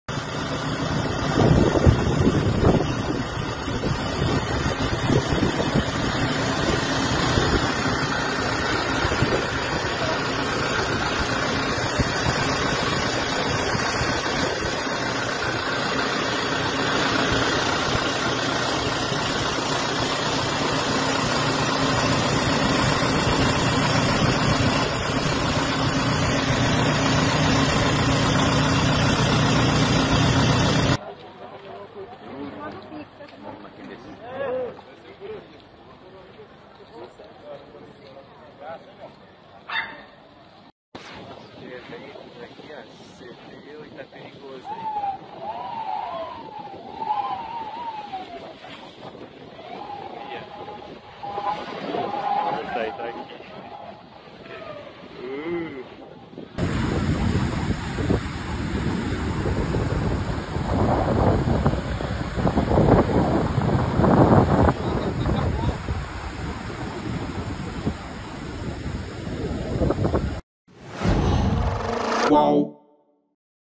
Chuvas no Litoral de São Paulo: Boiçucanga tem inundação e estradas e praias interditadas
As fortes chuvas no litoral paulista interditaram estradas e praias de Boiçucanga, em São Sebastião.